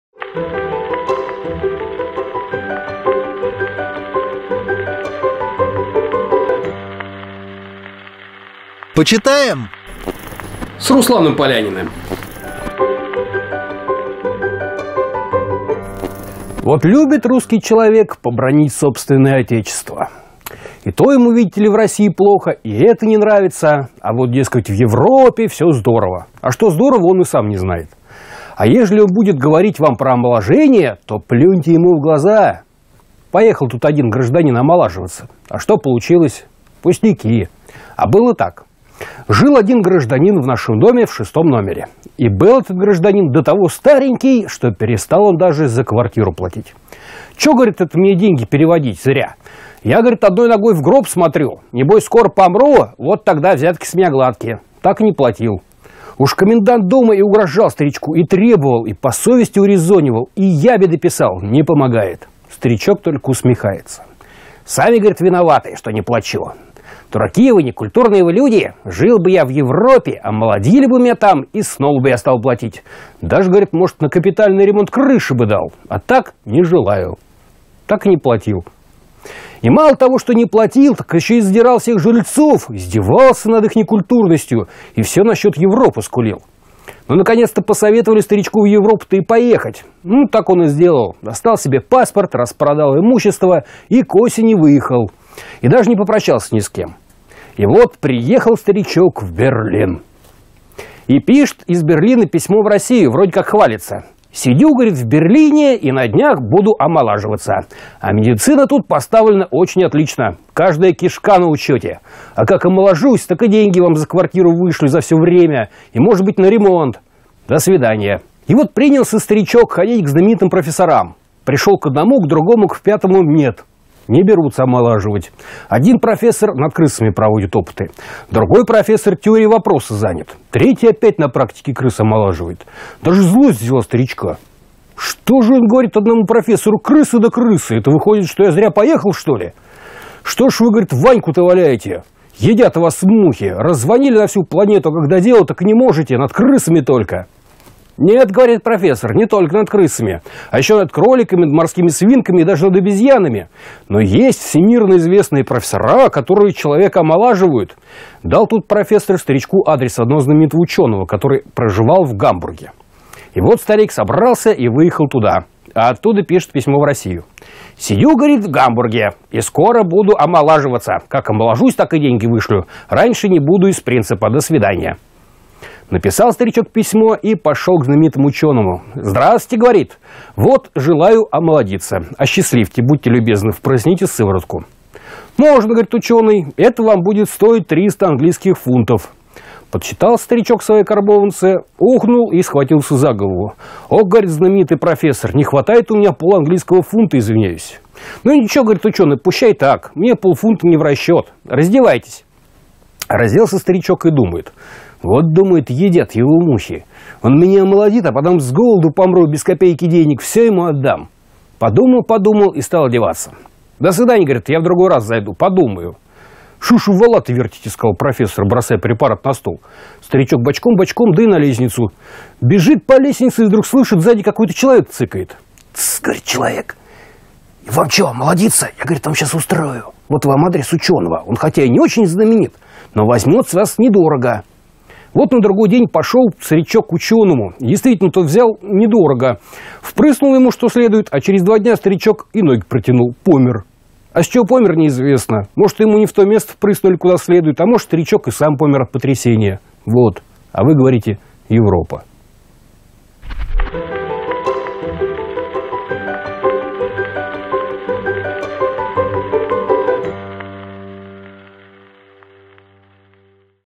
Наша новая рубрика «Почитаем?». И снова Зощенко в исполнении Руслана Полянина
Минутка хорошего настроения от Руслана Полянина. В рубрике с говорящим названием «Почитаем?» вспомним снова Михаила Зощенко и его произведение «Европа».